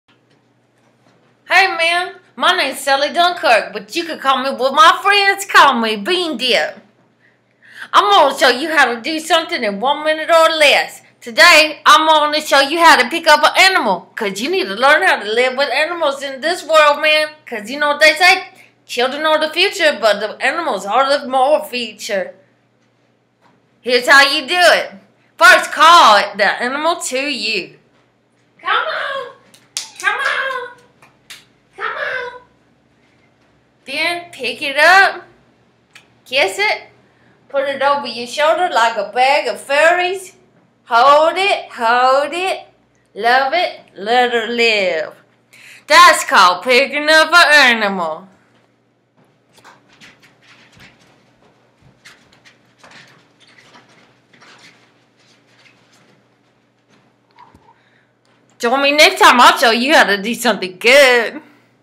Bean Dip Teaches You How To Do Stuff!!!!
This woman Sally Dunkirk (aka Bean Dip) lives a few houses down from me.  She asked me to upload the "how to" videos she's gonna start making.  I said yes.  (Partly out of fear)